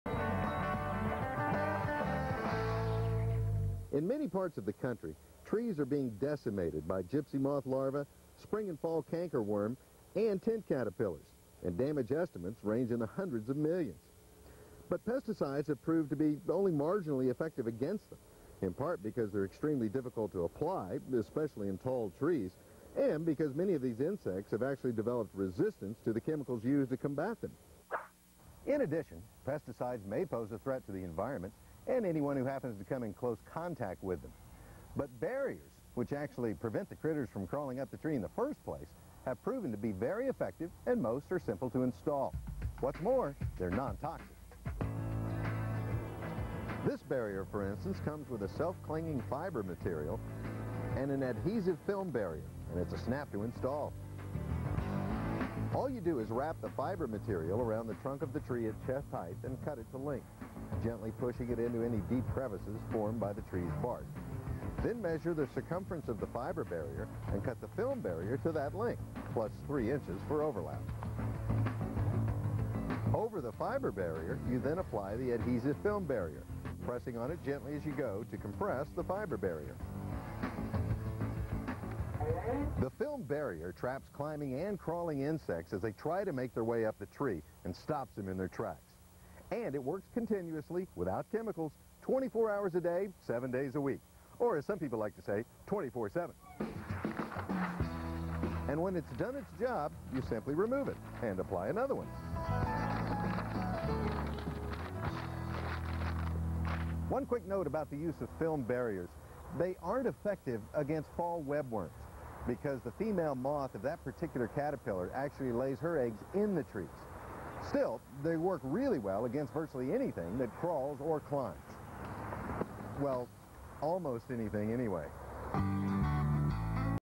BugBarrier HGTV
HGTV's Paul James demonstrates how the BugBarrier Tree Band is installed and how it protects trees from climbing pests like the Gypsy Moth.